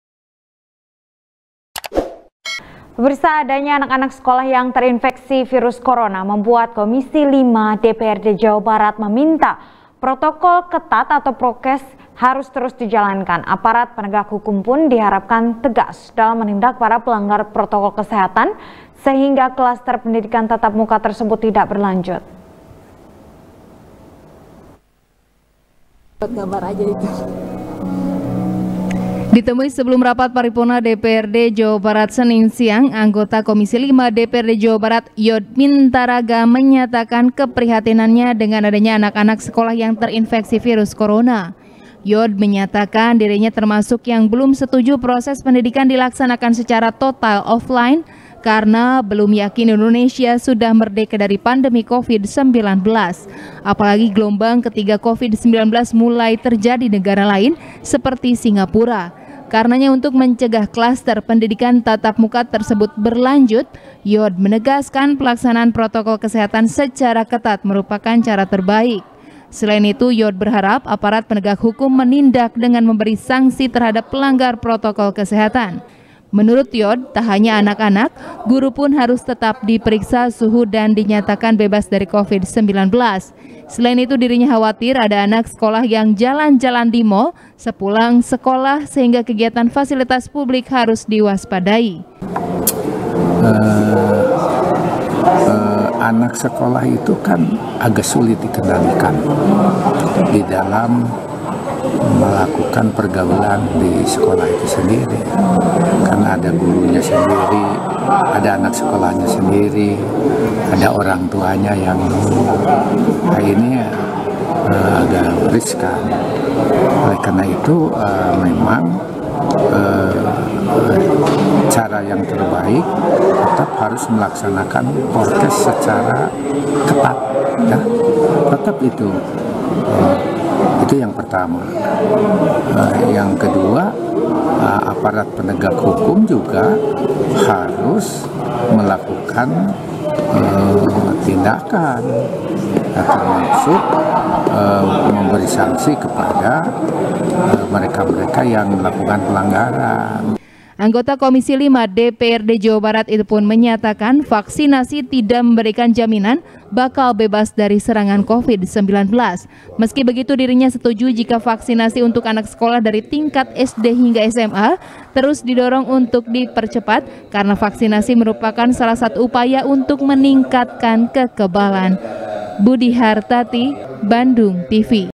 Klaster PTM, Komisi V Dprd Jabar Minta Prokes Ketat Dijalankan | SBR | BANDUNGTV
SBR | BANDUNGTV | NIKEN PRATIWI | Adanya AnakAnak Sekolah Yang Terinfeksi Virus Corona Membuat Komisi V DPRD Jawa Barat Meminta Protokol Kesehatan Atau Prokes Terus Ketat Dijalankan. Aparat Penegak Hukum Pun Diharapkan Tegas Dalam Menindak Para Pelanggar Protokol Kesehatan, Sehingga Klaster Pendidikan Tatap Muka Tersebut Tidak Berlanjut.